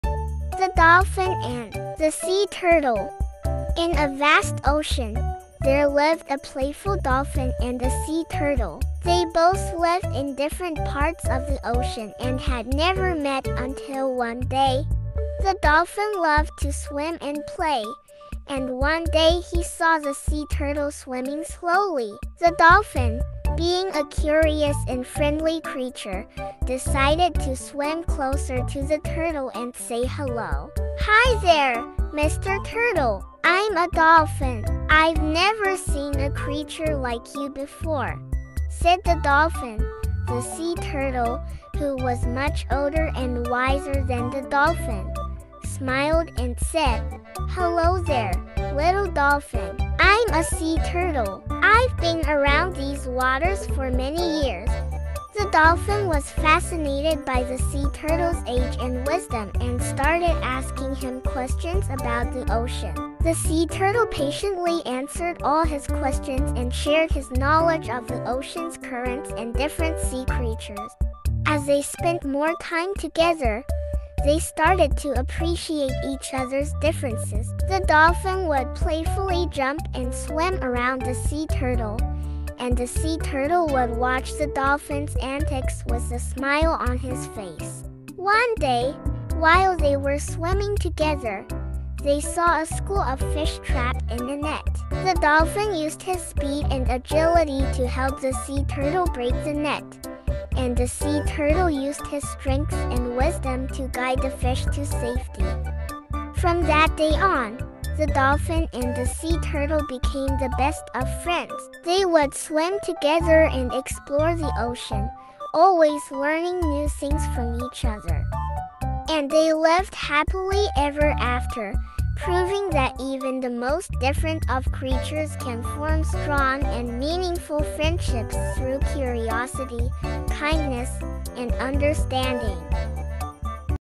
The Dolphin and the Sea Turtle | short story in English for kids | moral story
This is a heartwarming story about a playful dolphin and a sea turtle who become unlikely friends in the vast ocean. Despite their differences, they learn to appreciate each other's unique qualities and form a strong bond through their adventures and experiences. Through their friendship, they teach us the importance of kindness, curiosity, and understanding in forming meaningful relationships with others.